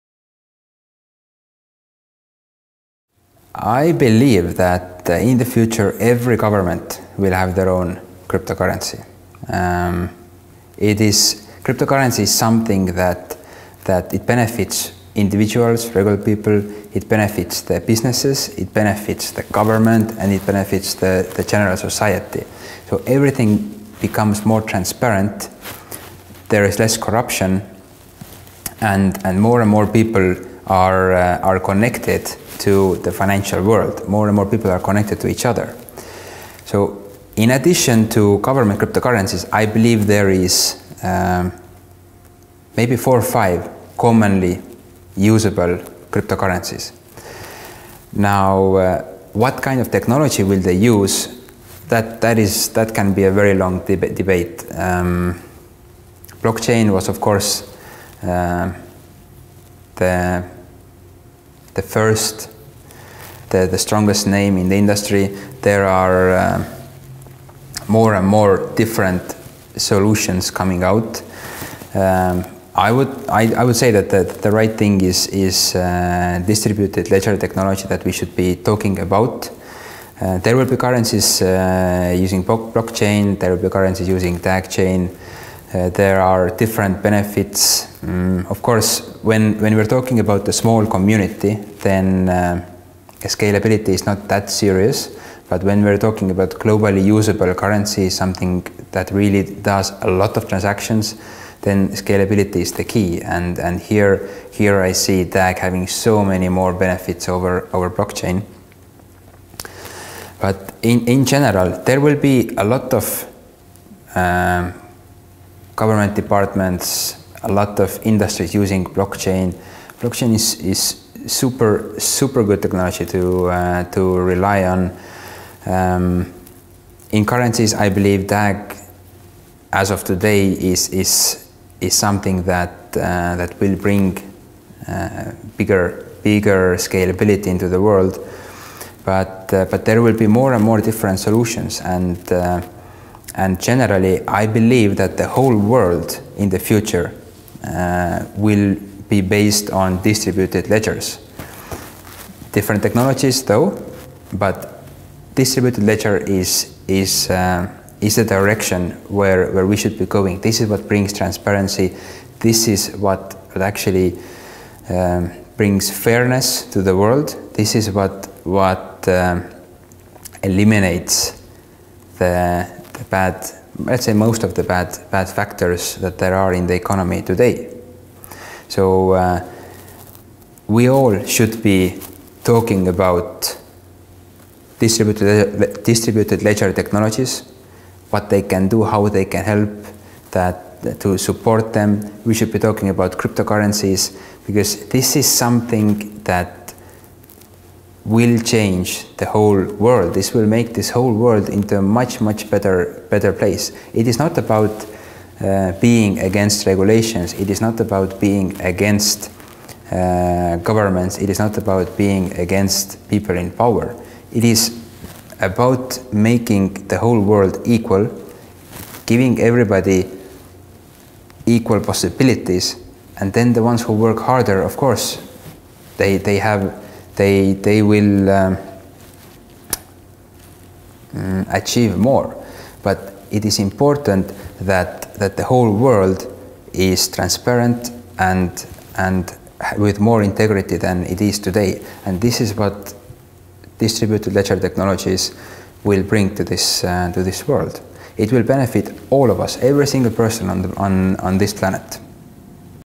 What is the Future of Cryptocurrencies? - (PART 5) DagCoin Explained - Success Factory Interview
Where do you see the future of cryptocurrencies?